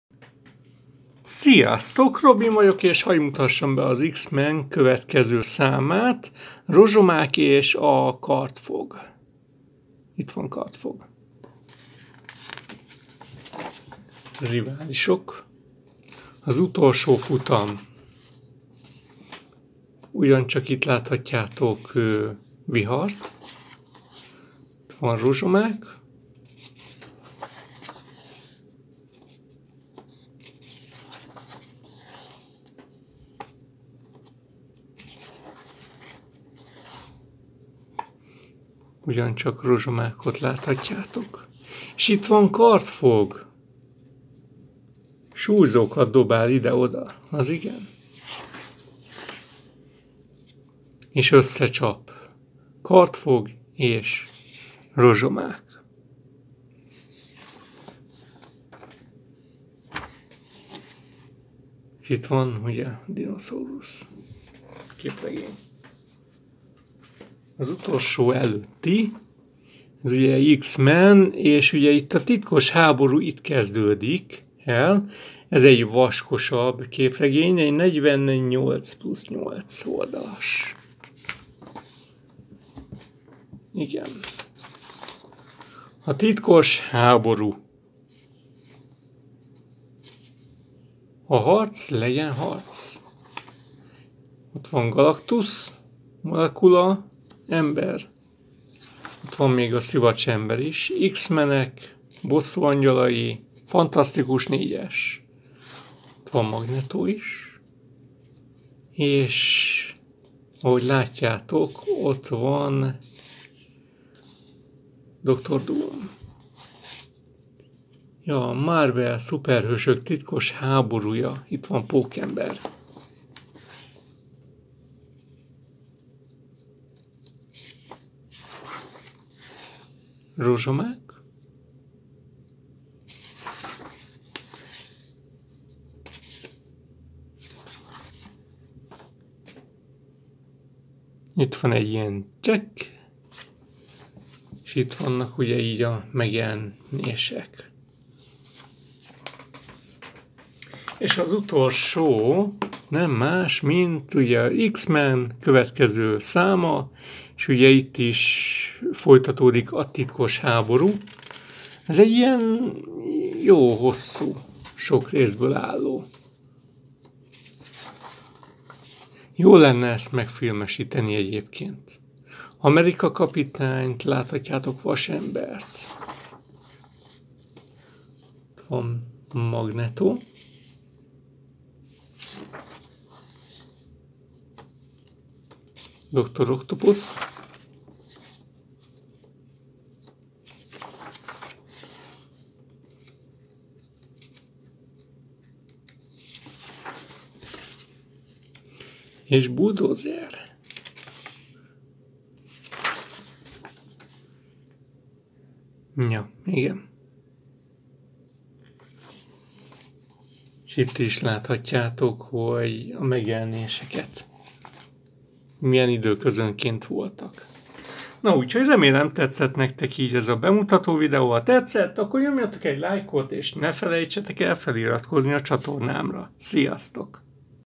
X-Men Képregények Bemutatása 4. Rész
Az X-Men egy, a Semic Interprint által kiadott képregénysorozat volt, mely 1992 és 1997 között jelent meg Magyarországon. A sorozat a Marvel Comics által kiadott Uncanny X-Men című képregénysorozat egyes történeteit tartalmazta.